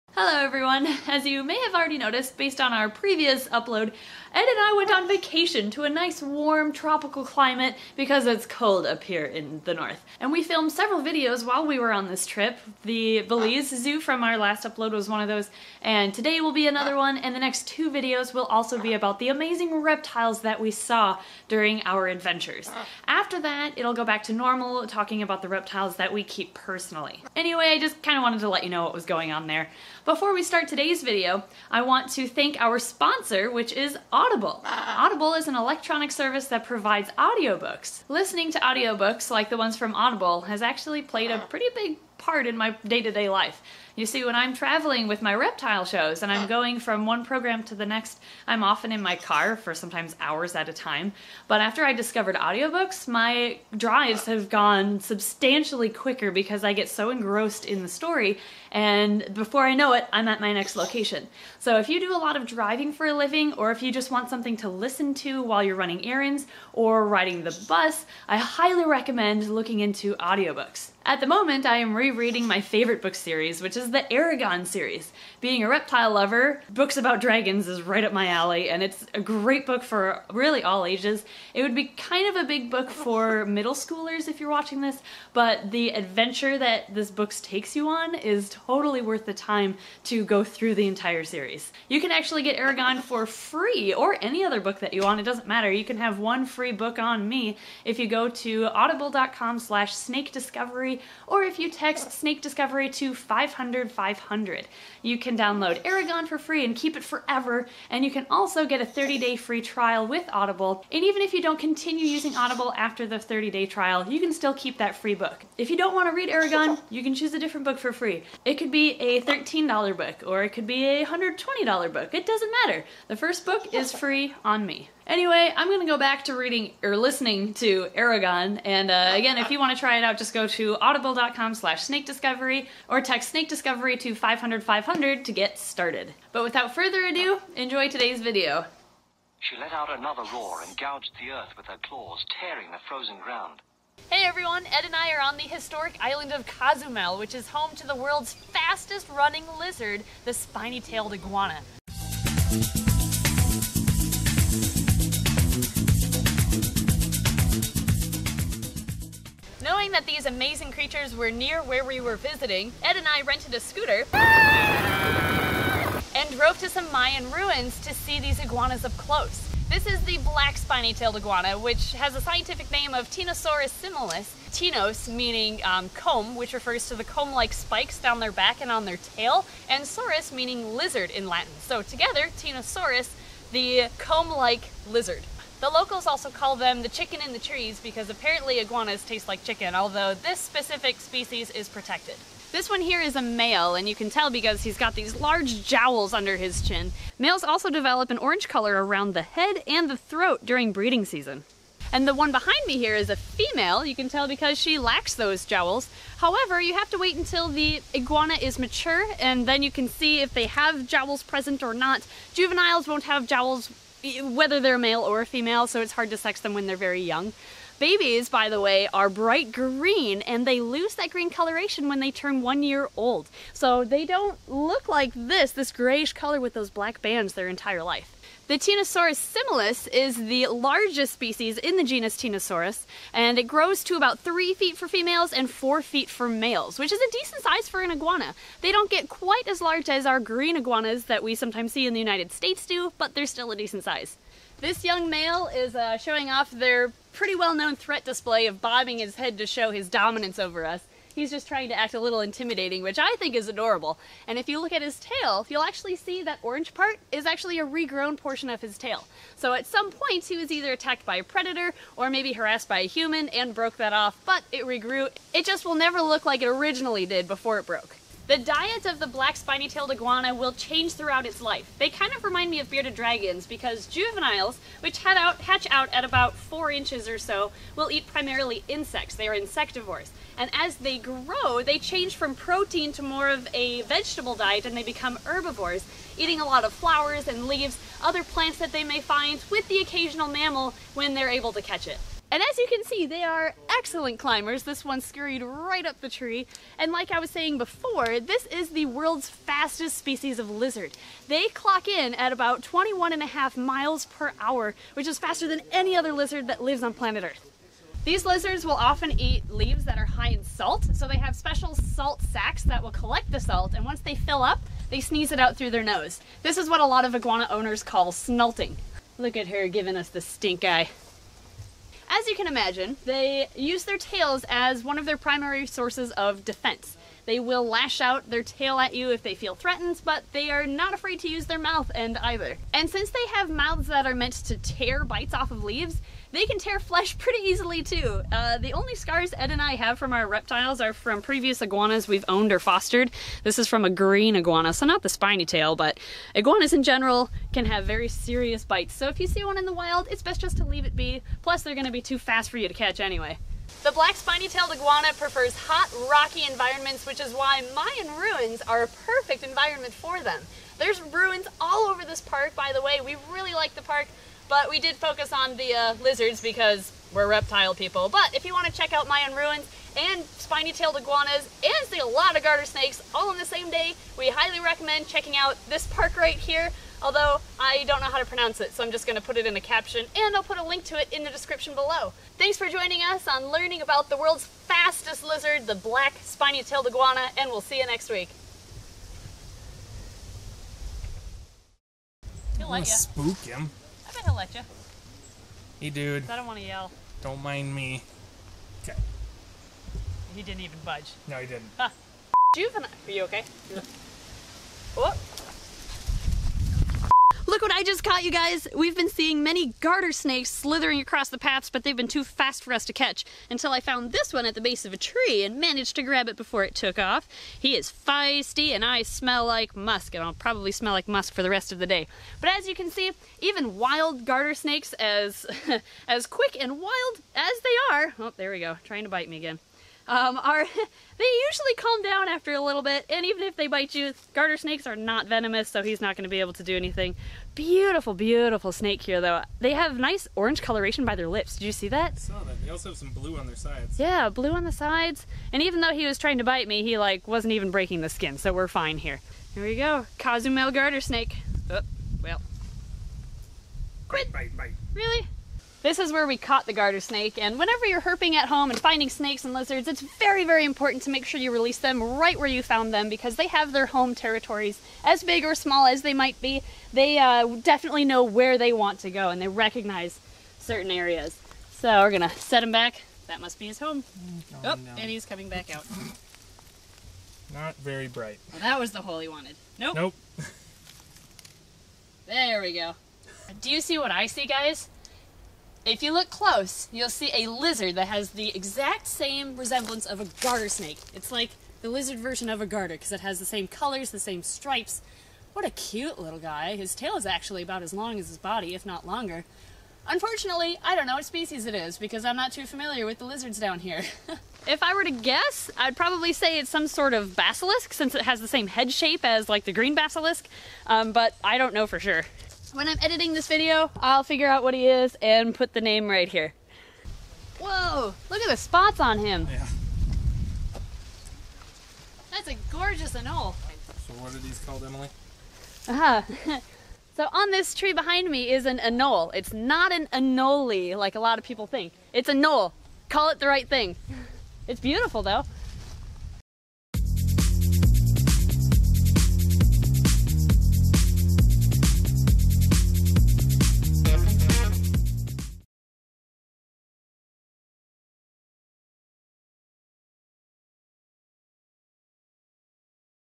Meet the Black Spiny Tailed Iguana!
Today we visit Cozumel to check out some wild black spiny tailed iguanas!! Learn about the world's fastest lizard in this video, from diet to behavior :)